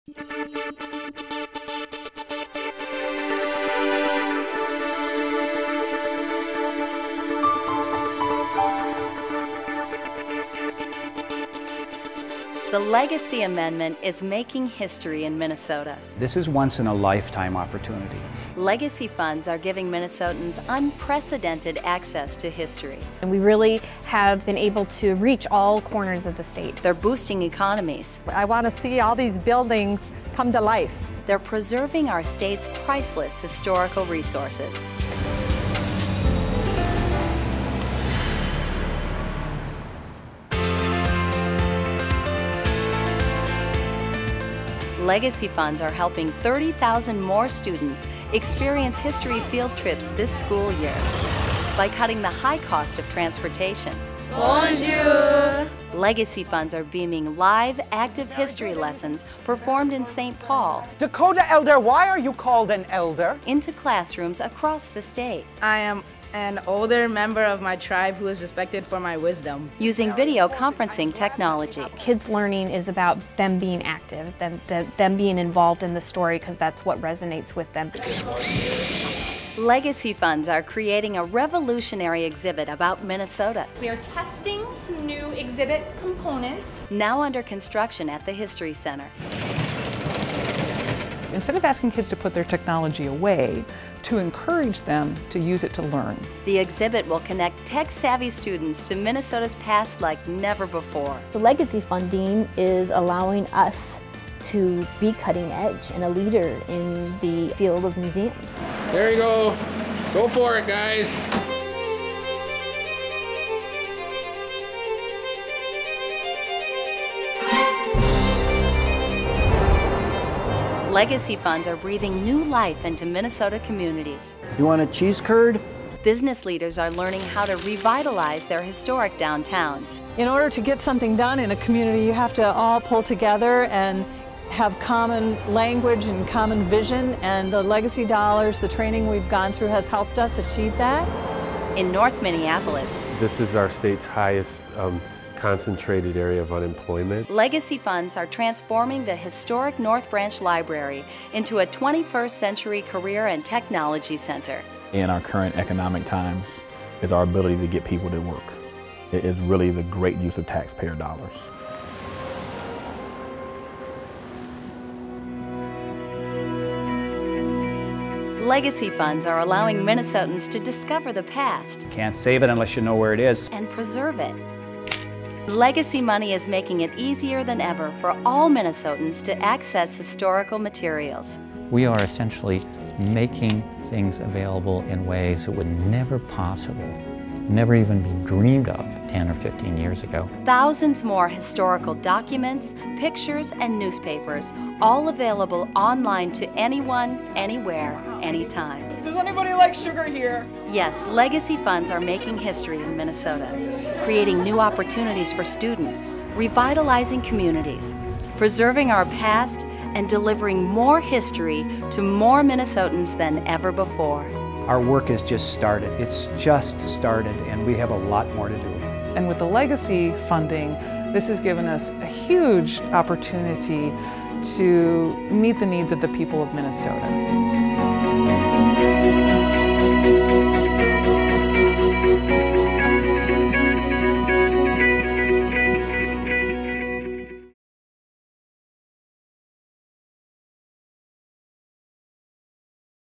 Minnesota History: Building a Legacy (2011)
Minnesota voters approved the Clean Water, Land and Legacy Amendment  in November, 2008. The Legacy Amendment supports outdoor heritage, clean water, parks and trails, as well as arts, history and cultural heritage. It has created an unprecedented opportunity for the Minnesota Historical Society to preserve and share our state's rich heritage.